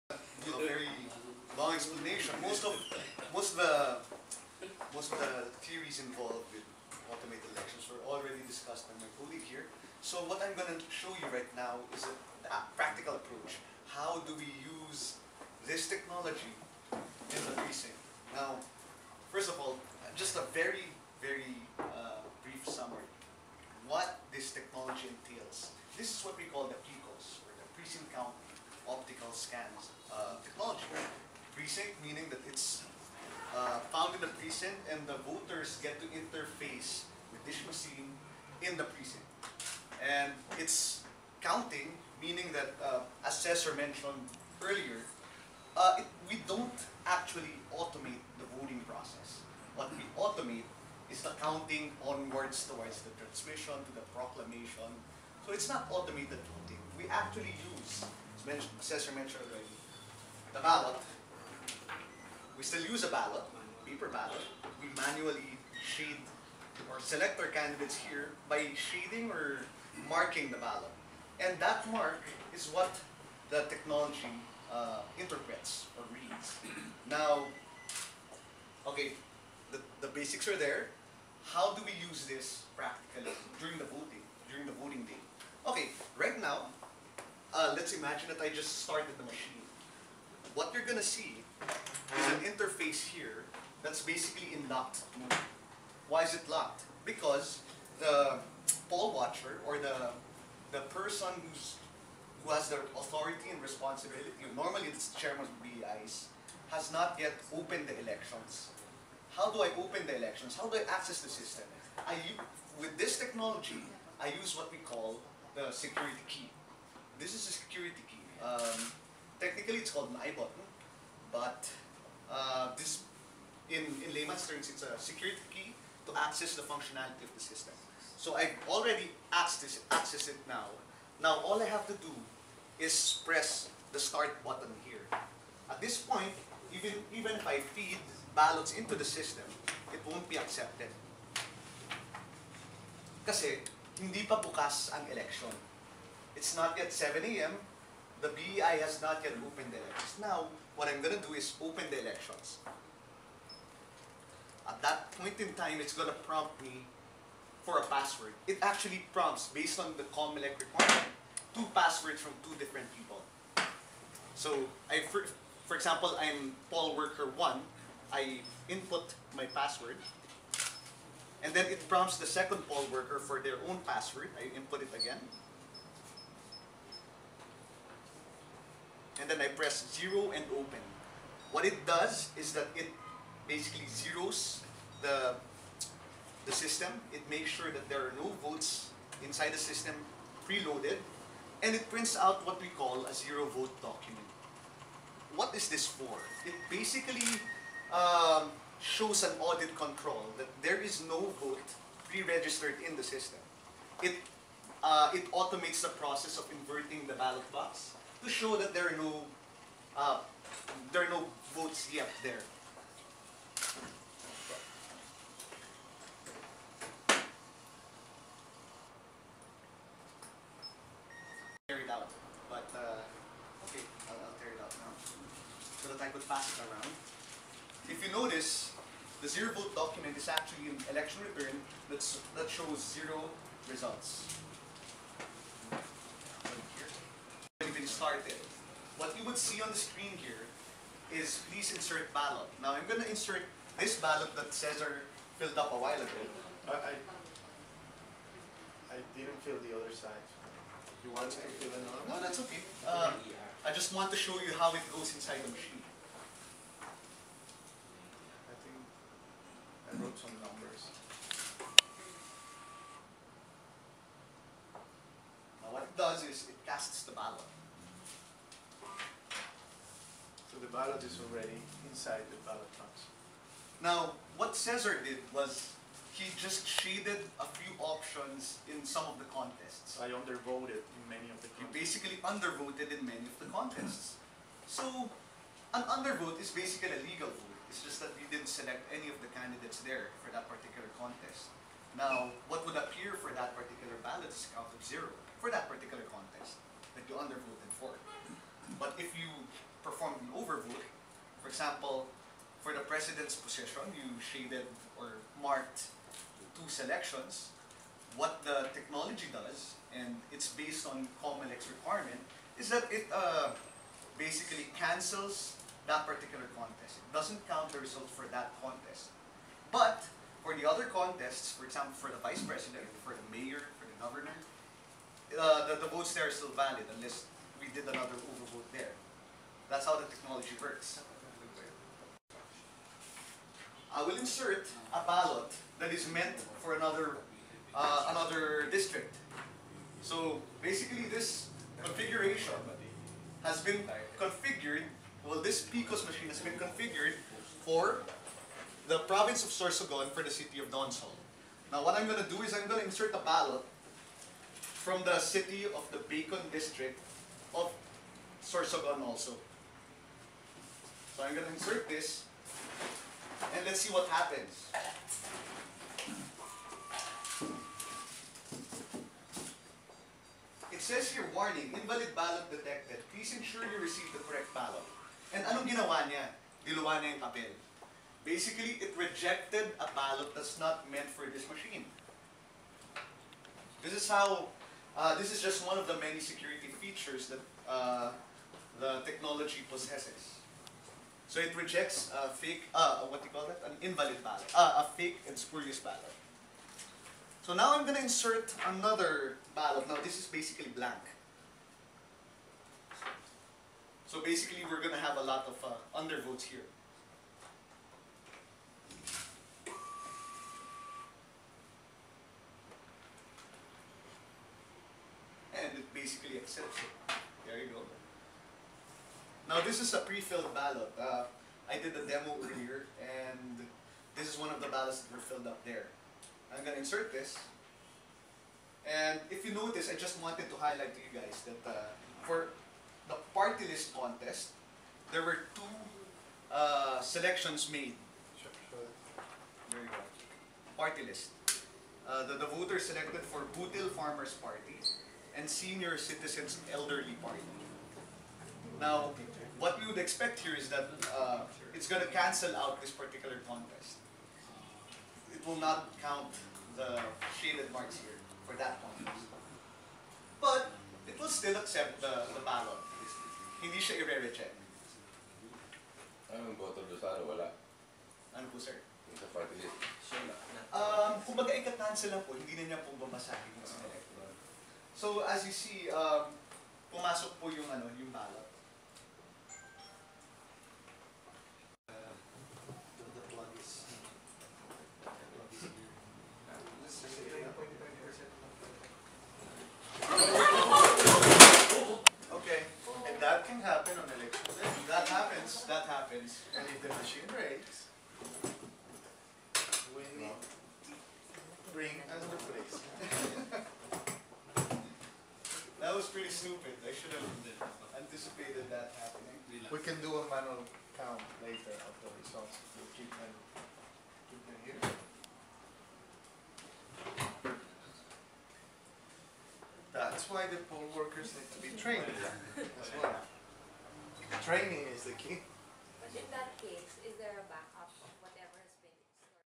Automated Elections by Smartmatic
Smartmatic executives visited the Philippine Daily Inquirer office last night, July 3, 2009 and demonstrated to editors and reporters the process of voting using their computer.